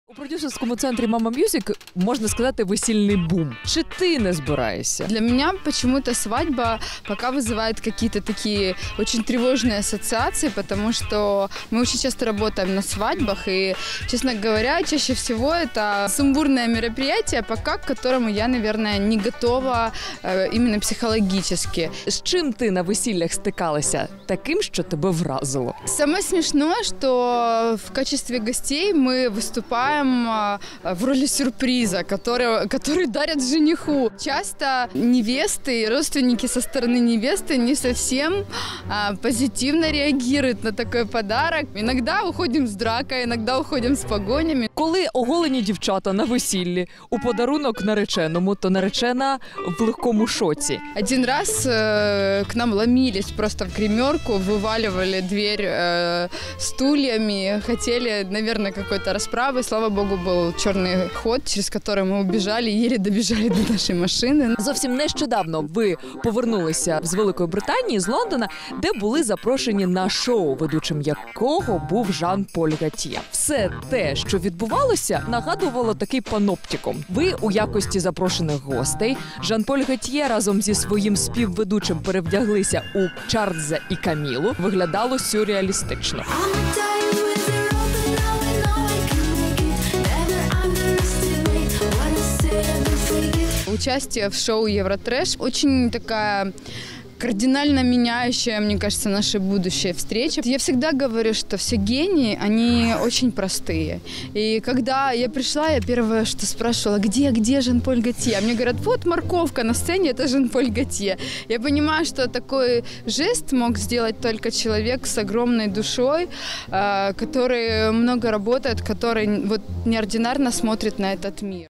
Дар’я Астаф’єва танцювала з Жаном-Полем Готьє у Лондоні
Співачку Дар’ю Астаф’єву та гурт NIKITA запросили до Великобританії взяти участь у шоу Eurotrash, ведучим якого є Жан-Поль Готьє. Астаф’єва поділилась своїми враженнями від зустрічі із легендарним французьким модельєром, який не соромився танцювати у кумедному костюмі.